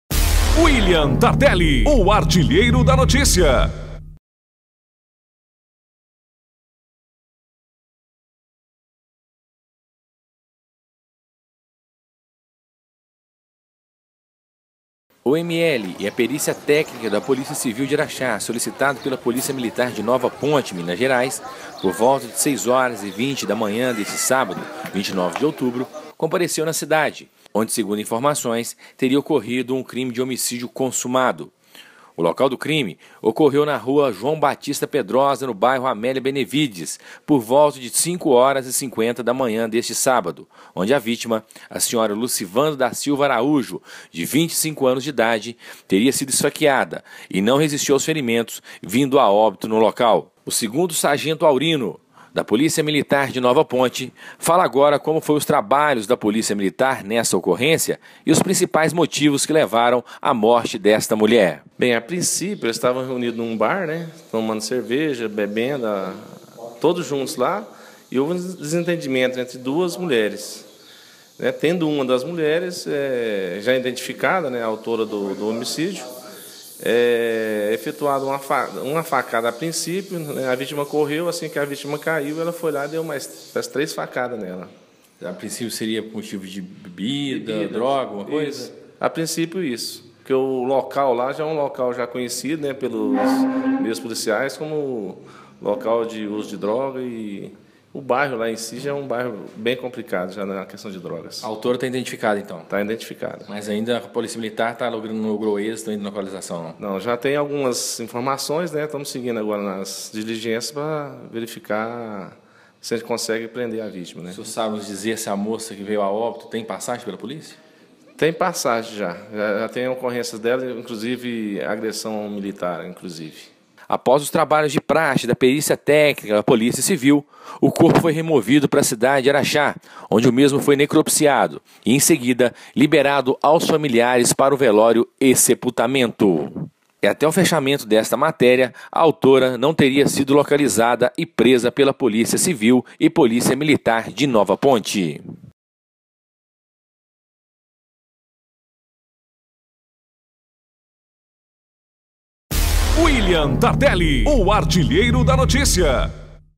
Mulher de 25 anos é morta a golpes de faca, após discutir com outra mulher em Nova Ponte
O IML e a Perícia Técnica da Policia Civil de Araxá, solicitado pela Policia Militar de Nova Ponte/MG, por volta de 06h20, da manhã de sábado, 29 de outubro compareceu na cidade, onde segundo informações teria ocorrido um crime de homicídio consumado.

O local do crime ocorreu na rua João Batista Pedrosa, no bairro Amélia Benevides, por volta das 05:50 desta mesma de sábado, onde a vítima LUCIVANDA DA SILVA ARAUJO, de 25 anos, teria sido esfaqueada e não resistiu aos ferimentos, vindo a óbito no local.

Segundo a Policia Militar a vítima estava em um bar realizando uso de bebida alcoólica com um grupo de pessoas, e neste local se encontrava a autora Naidé Aparecida dos Reis, 29 anos, que por motivos ainda desconhecidos, entrou em desentendimento com a vítima e começaram a discutir.

No calor das discussões a autora Neidé Aparecida, desferiu uma facada na vítima, que ainda chegou a caminhar por alguns metros e caiu, a autora não satisfeita se aproximou da vítima e ainda efetuou mais algumas facadas na vítima, que não resistiu aos ferimentos e veio a o óbito no local.

O Corpo da vítima foi removido para o IML de Araxá, após os trabalhos da perícia técnica da Policia Civil. Rastreamentos seguem no intuito de localizar e prender a autora do crime que ainda se encontra foragida, ate o fechamento desta matéria, as 23h20 da noite de sábado, 29 de outubro, a mesma não tinha sido localizada.